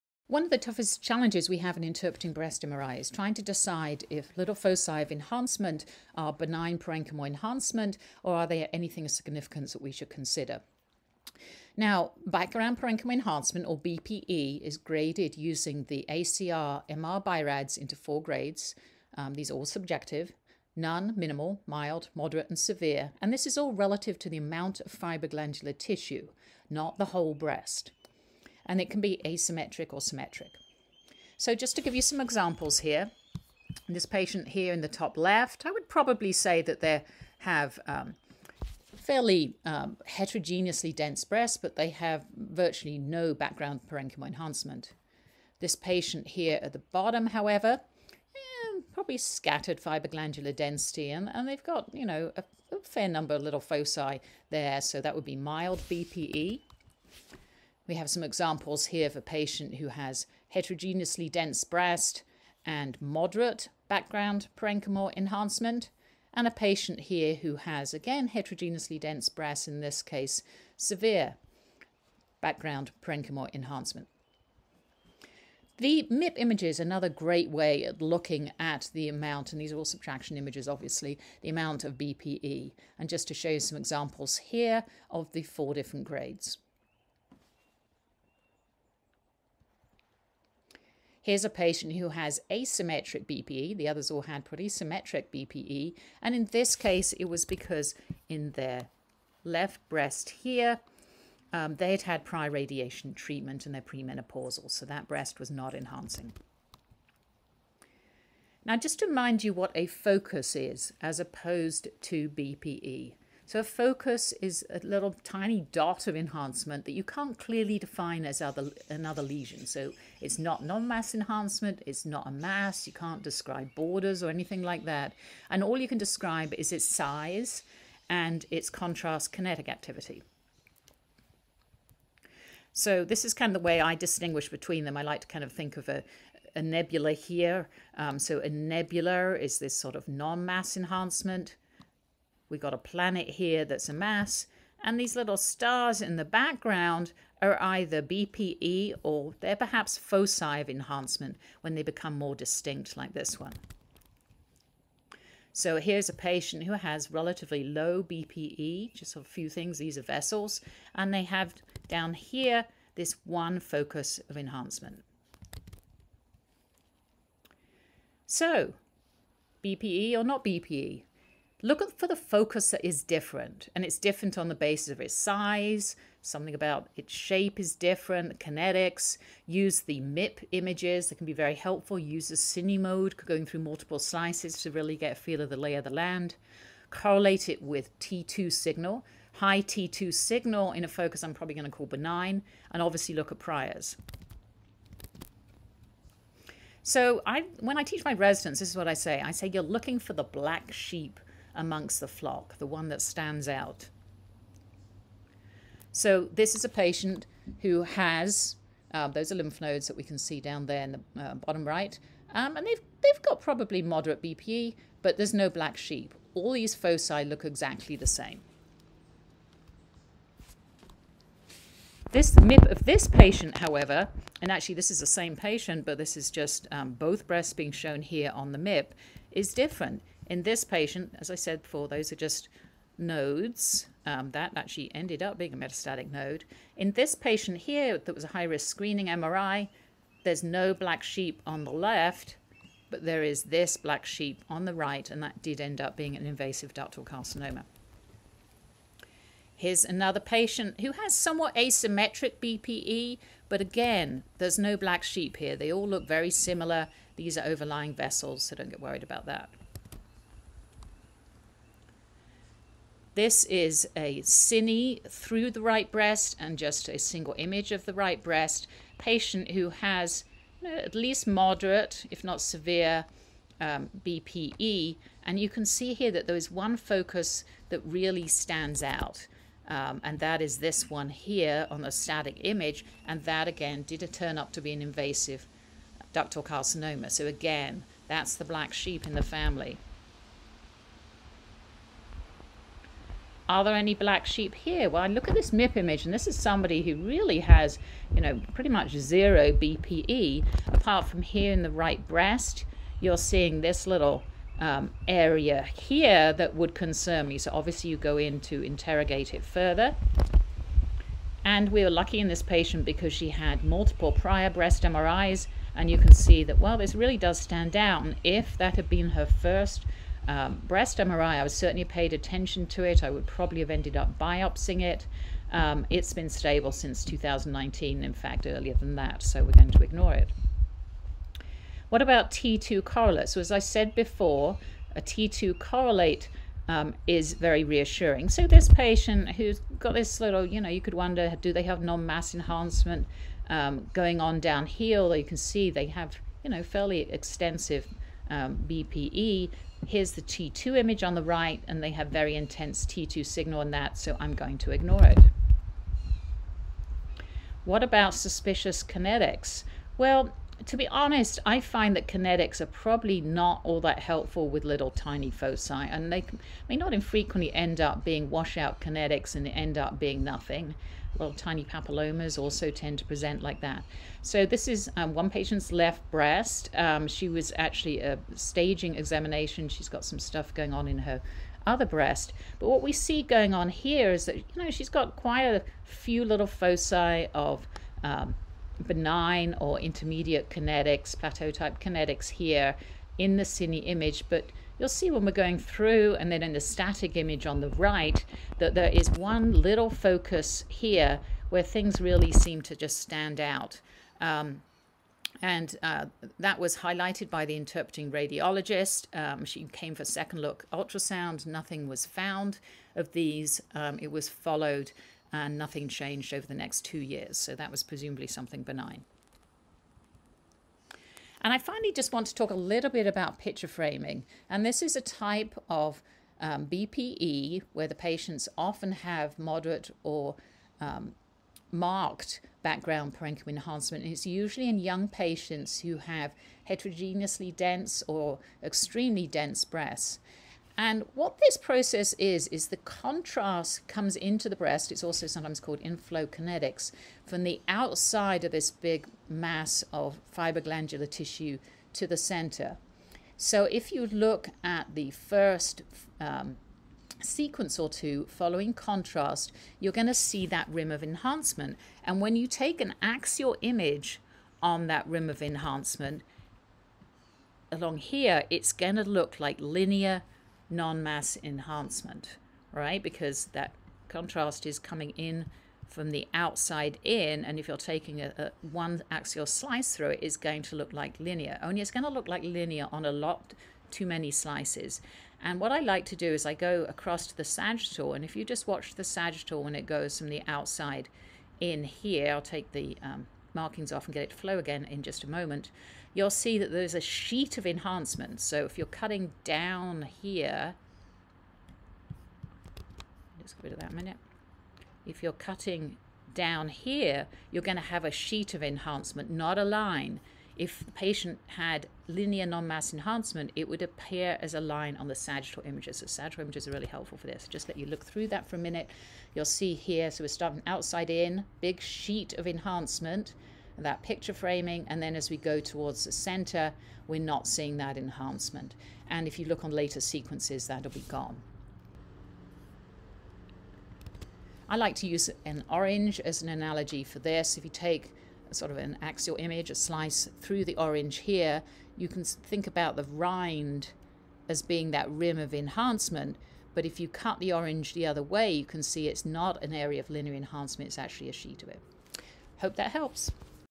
BPE or not BPE?
It's often tough when reading breast MRI scans to decide if a focus of enhancement is part of the benign parenchymal enhancement of this patient or something to be concerned about.  I describe the thought process I use when interpreting these studies.